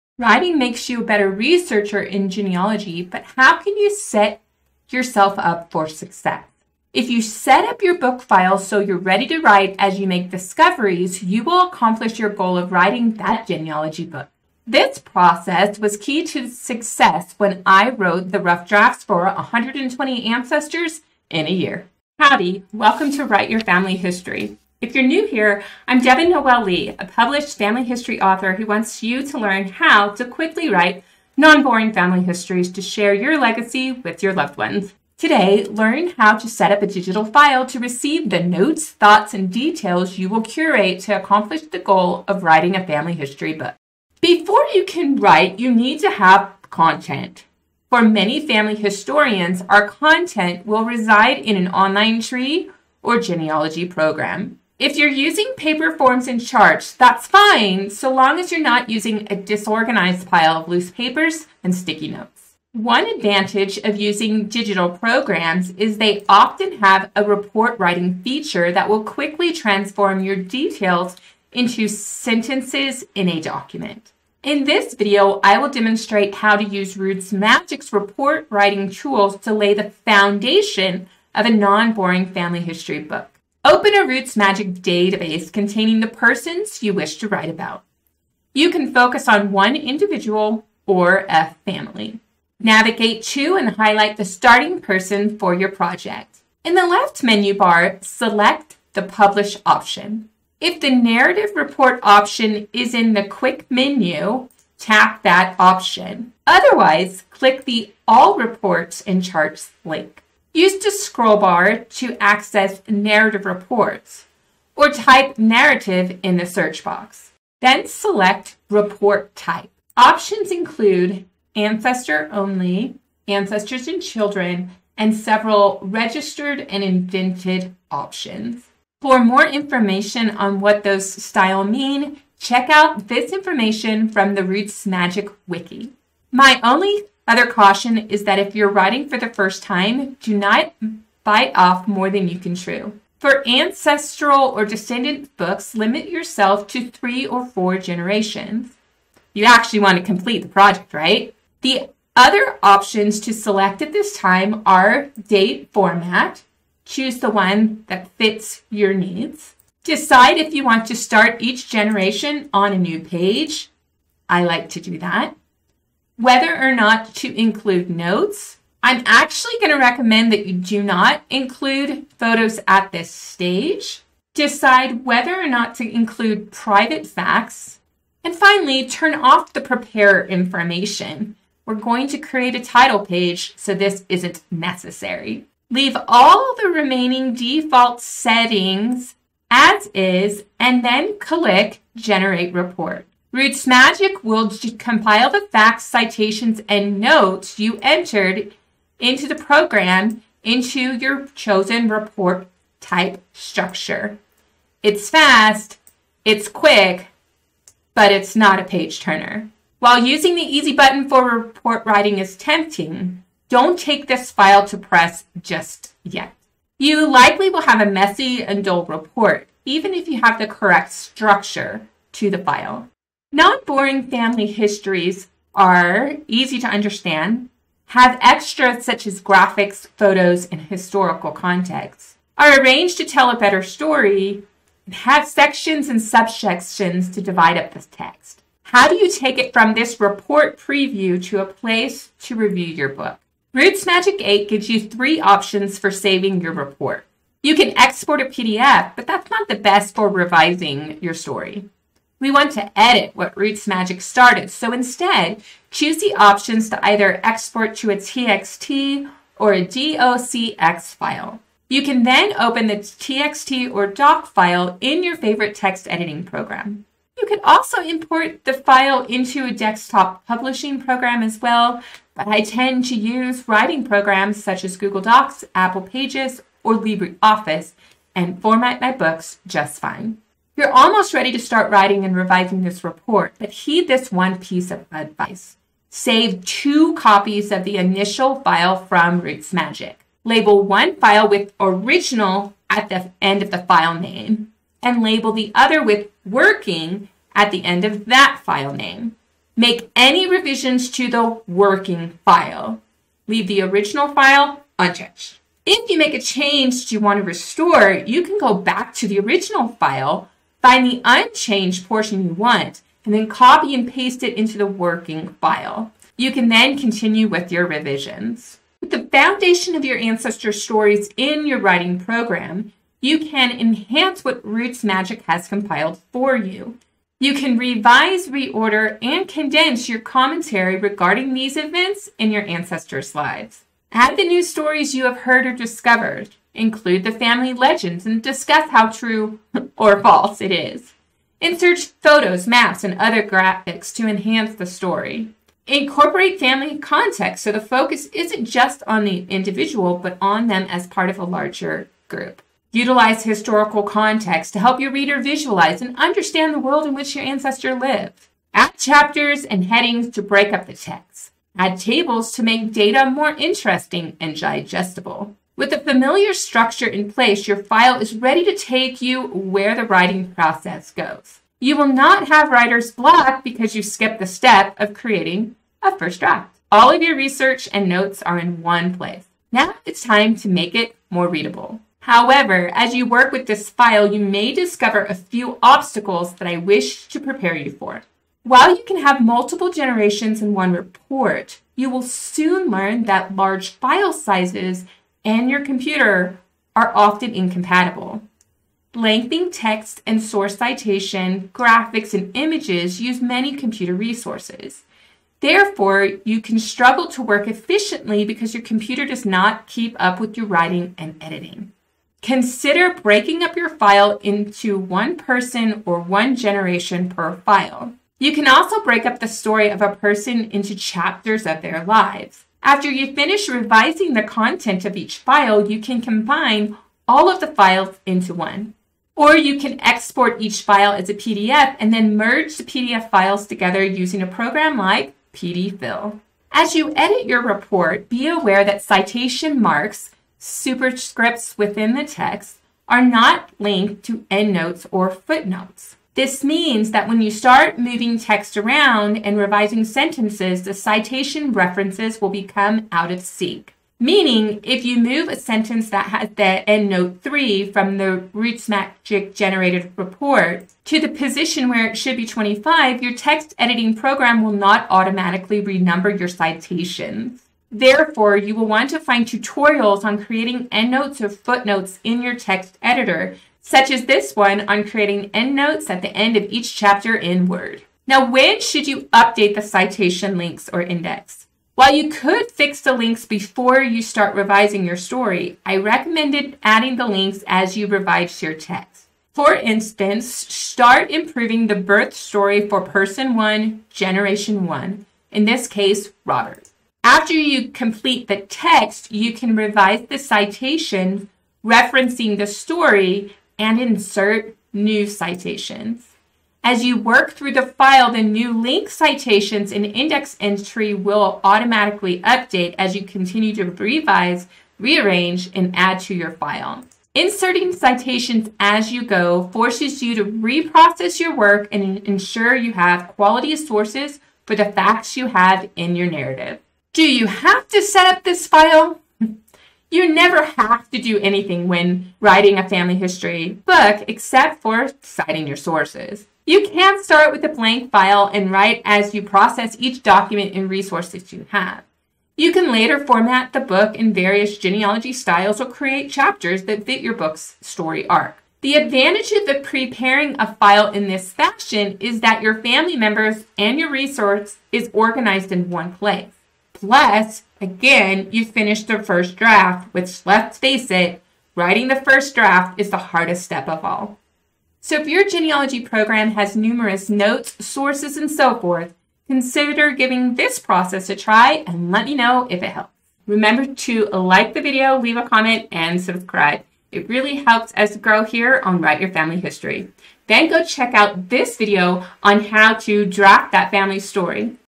QUICKLY Create a Draft For Genealogy Book With RootsMagic​
Set up a file to write a non-boring family history by starting with Narrative Reports on @RootsMagicTV. Then import the file to your writing program, and you're ready.

CONTINUE LEARNING

📗 Would you like to learn the recipe for writing family histories?

CHAPTERS

00:00 Introduction
00:43 Before You Begin
01:23 Generate Narrative Report with RootsMagic
03:29 Don’t Publish Narrative Report
03:58 Save RootsMagic Narrative Report for Editing
04:40 Make Two Copies of the File
05:20 Revise Working File
06:27 Writing File Problems
08:03 When to Update Source Citations
08:52 Do You Have to Set Up this File?